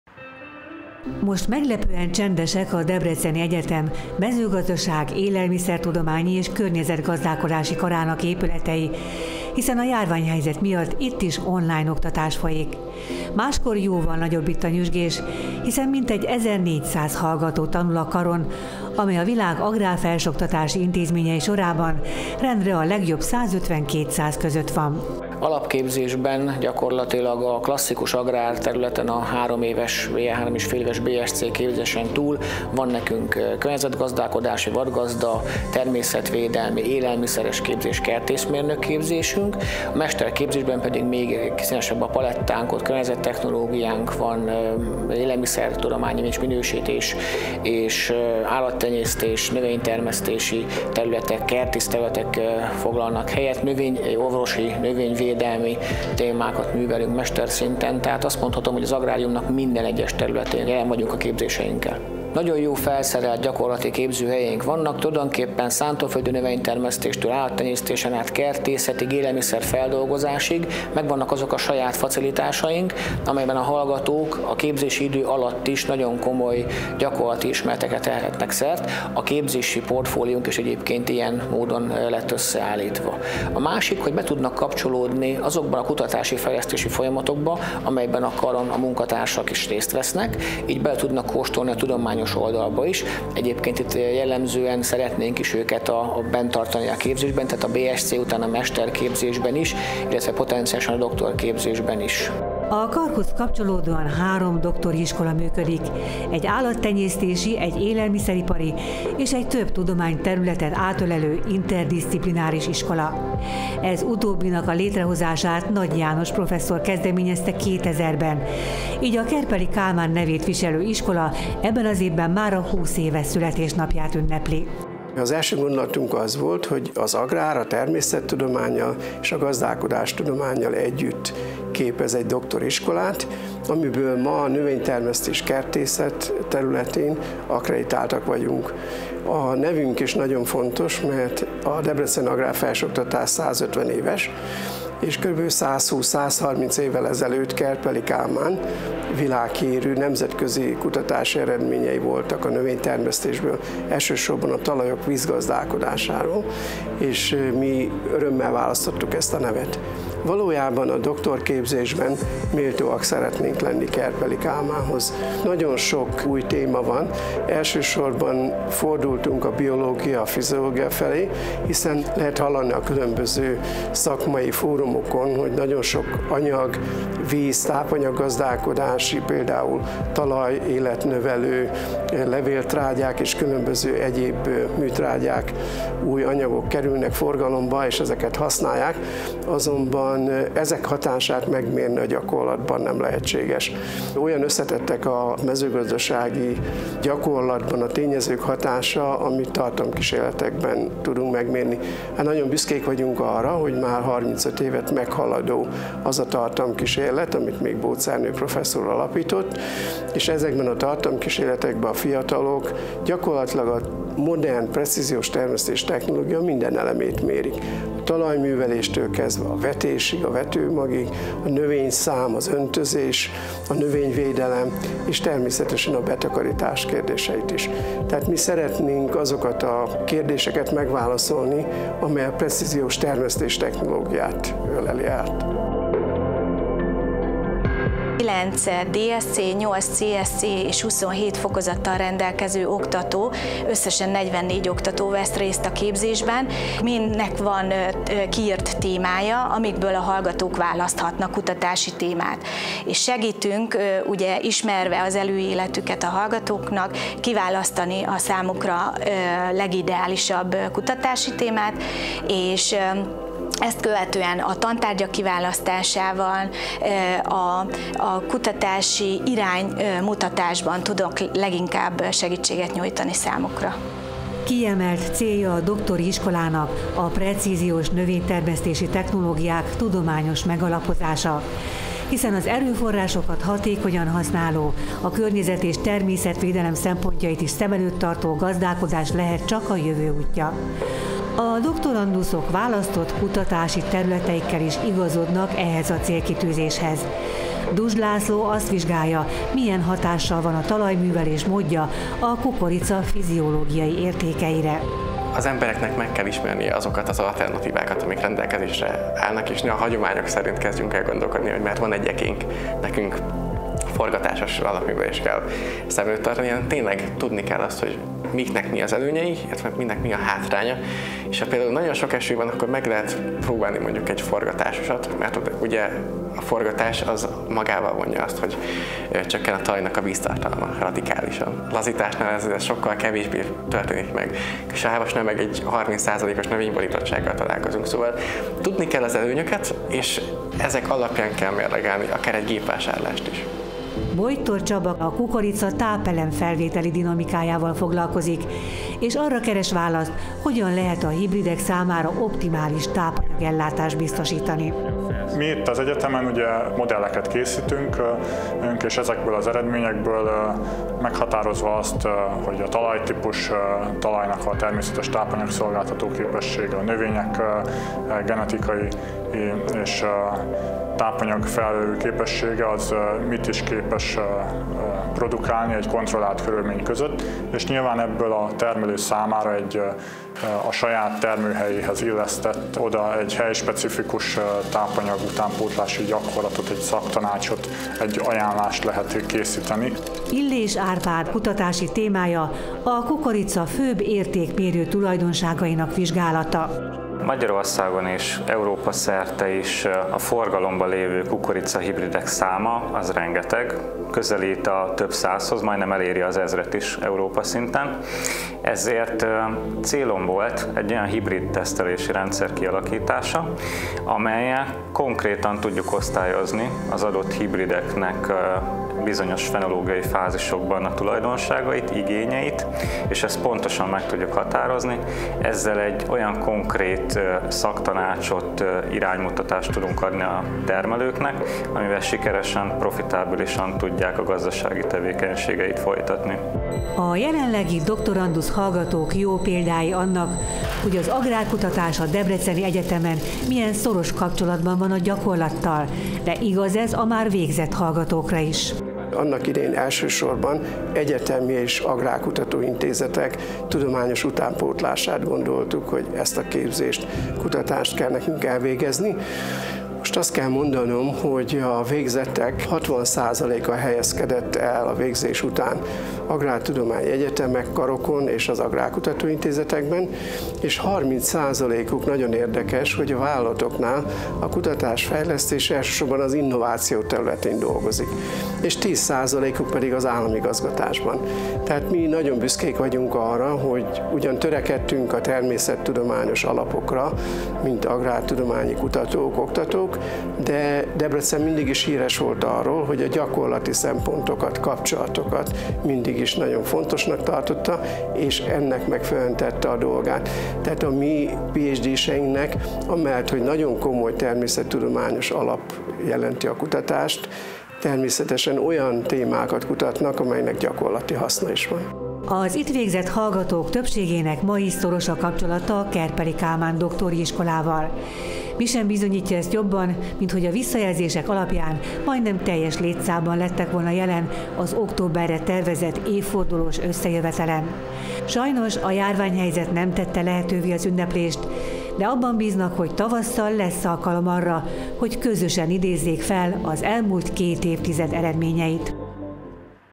Agrár doktorandusz képzés Debrecenben
Dr. Stündl László - dékán, Debreceni Egyetem Mezőgazdaság-, Élelmiszertudományi és Környezetgazdálkodási Kar
Dr. Nagy János - vezető, Kerpely Kálmán Doktori Iskola
Dr. Kakuszi-Széles Adrienn - intézetvezető, DE MÉK Földhasznosítási, Műszaki és Területfejlesztési Intézet
Duzs László - doktorandusz, Kerpely Kálmán Doktori Iskola
Illés Árpád - doktorandusz, Kerpely Kálmán Doktori Iskola
Bojtor Csaba - doktorandusz, Kerpely Kálmán Doktori Iskola